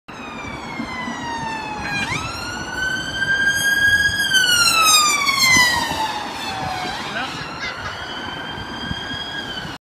Scottish Ambulance Service (SAS)
SAS seen responding blue along Maxwellton Street.

Feel free to follow me on Flickr Haggisspotter999.